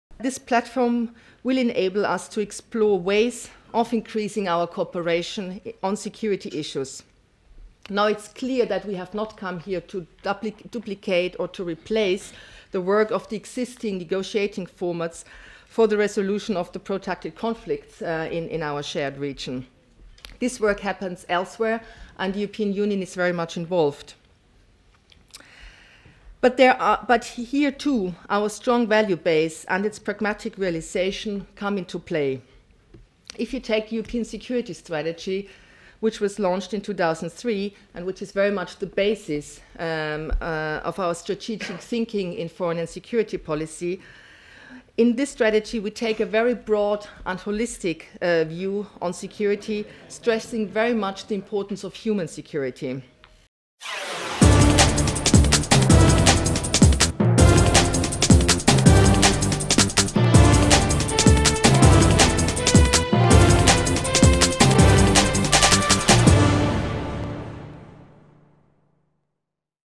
Eastern Partnership Conference: Helga Schmid
Helga Schmid, a senior advisor on relations between the EU and countries on the easters borders of EU member states, talks about the Eastern Partnership at a conference in Brussels.